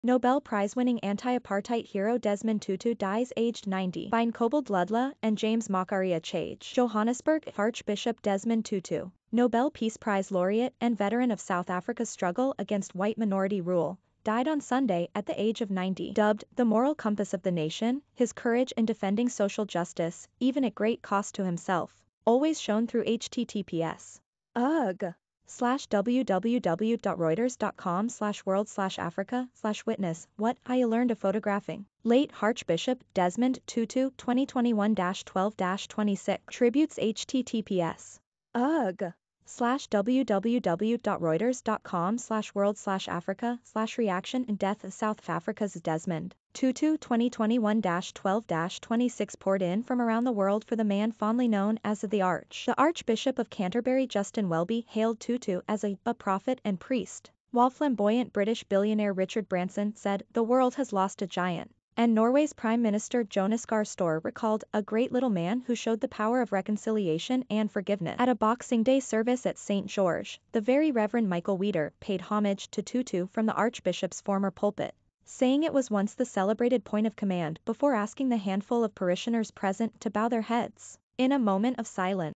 Nobel Prize-winning anti-apartheid hero Desmond Tutu dies aged 90
Nobel Prize-winning anti-apartheid hero Desmond Tutu dies aged 90 By Nqobile Dludla and James Macharia Chege JOHANNESBURG -Archbishop Desmond Tutu, Nobel Peace Prize laureate and veteran of South Africa's struggle against white minority rule, died on Sunday at the age of 90. Dubbed "the moral compass of the nation", his courage in defending social justice, even at great cost to himself, always shone through Tributes poured in from around the world for the man fondly known as "The Arch". The Archbishop of Canterbury Justin Welby hailed Tutu as a "a prophet and priest" while flamboyant British billionaire Richard Branson said "the world has lost a giant" and Norway's Prime Minister Jonas Gahr Stoere recalled "a great little man who showed the power of reconciliation and forgiveness". At a Boxing Day service at St George’s, the Very Reverend Michael Weeder paid homage to Tutu from the Archbishop's former pulpit, saying it was “once the celebrated point of command” before asking the handful of parishioners present to bow their heads in a moment of silence.